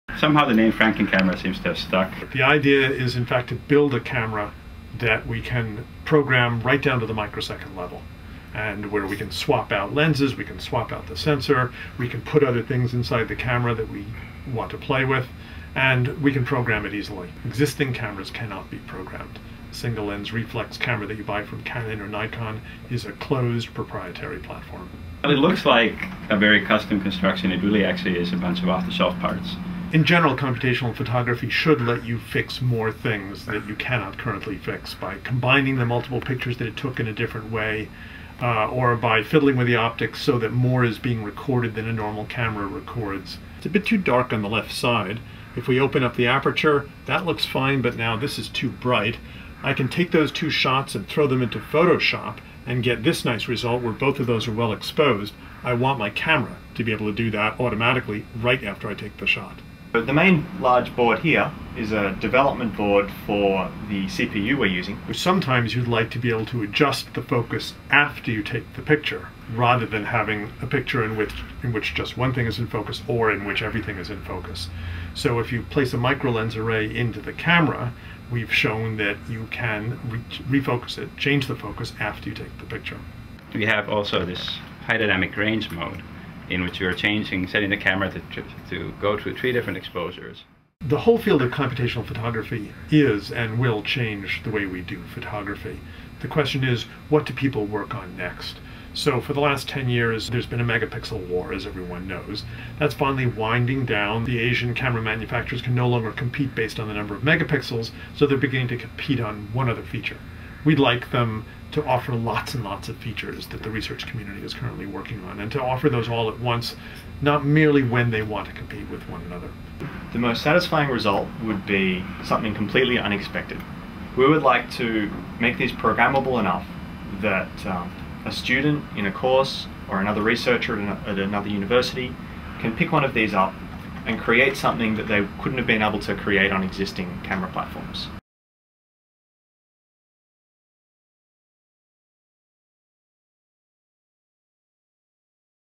Stanford Photo Scientists are Teaching the Camera New Tricks
Stanford scientists have created an open-source camera that could revolutionize digital photography by giving programmers that chance to design software to teach the device new tricks.